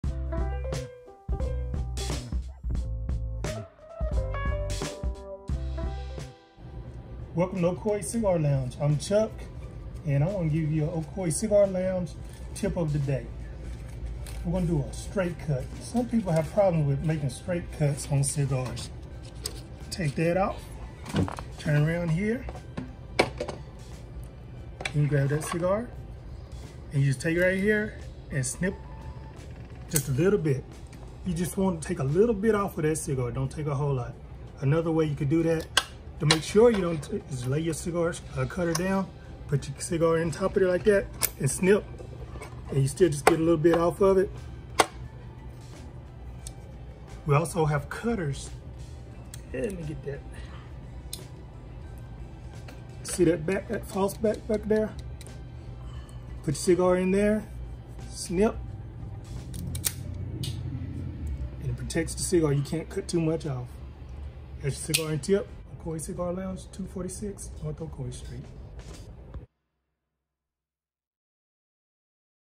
Cigar Tips 101-Pt 5 | The Guillotine cut
Chuck shows us how to properly do a straight cut and not take too much off the top.